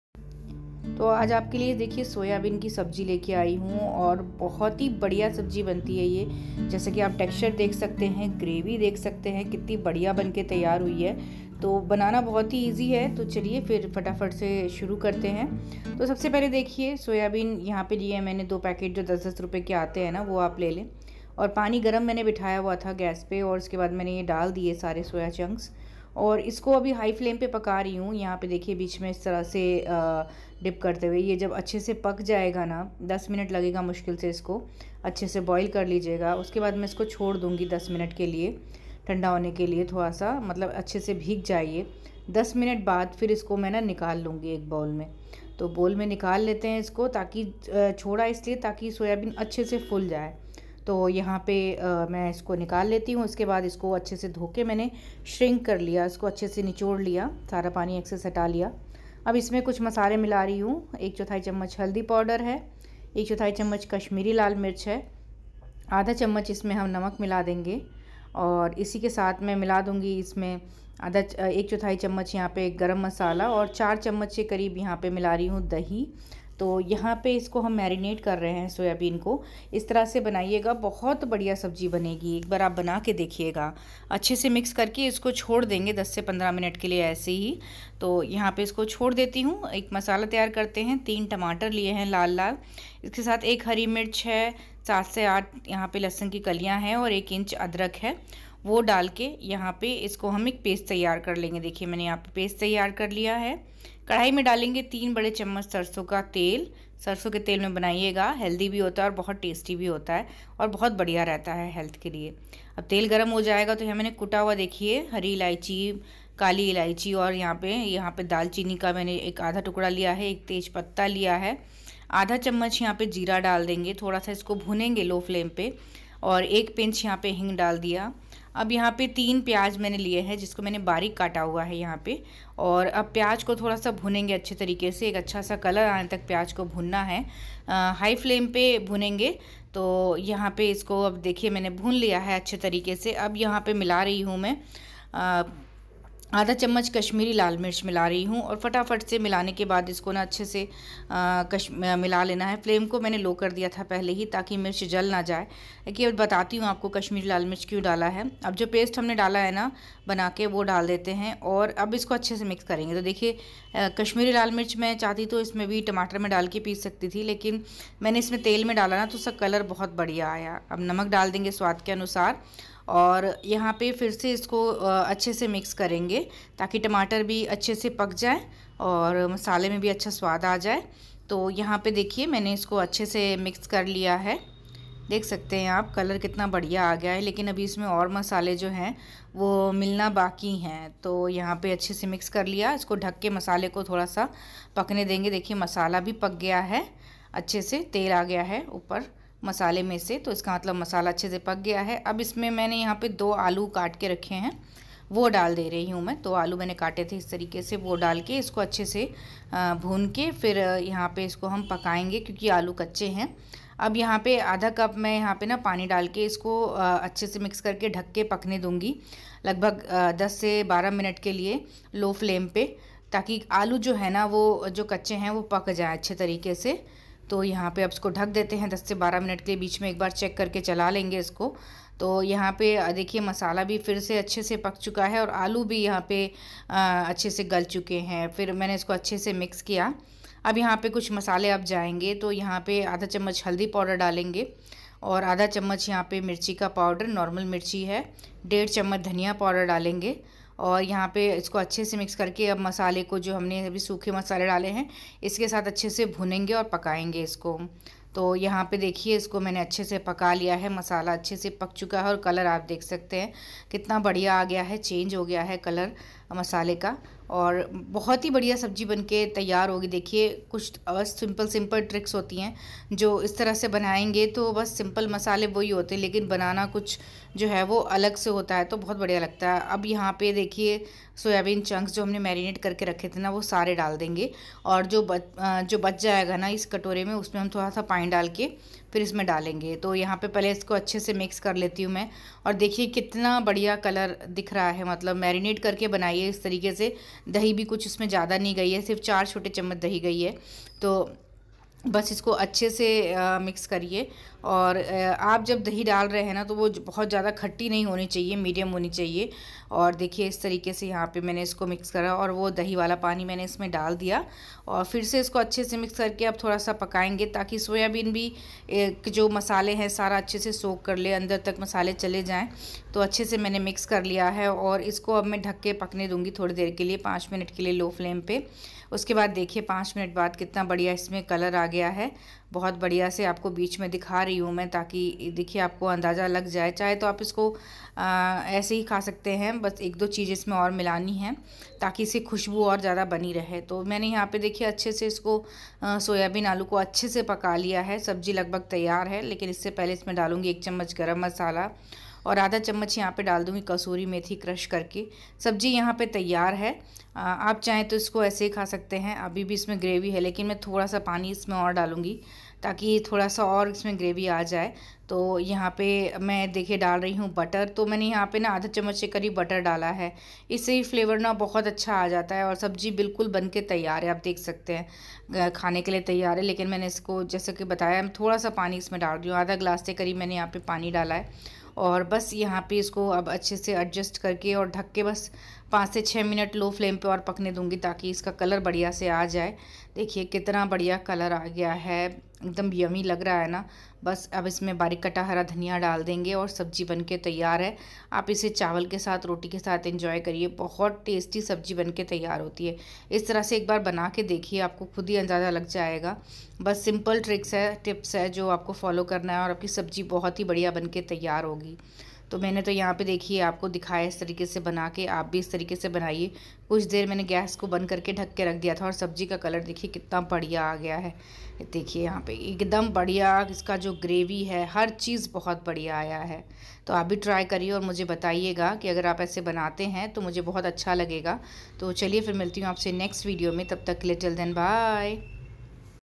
सोयाबीन की सब्ज़ी अगर ऐसे बनाओगे तो उंगलियां चाटते रह जाओगे ||Soyabean Ki Sabji
**Description:**
Soyabean Ki Sabji, when prepared this way, will have you licking your fingers! This flavorful and nutritious dish is packed with protein and spices, making it a delicious and healthy option for any meal. It's perfect for those looking for a tasty vegetarian alternative.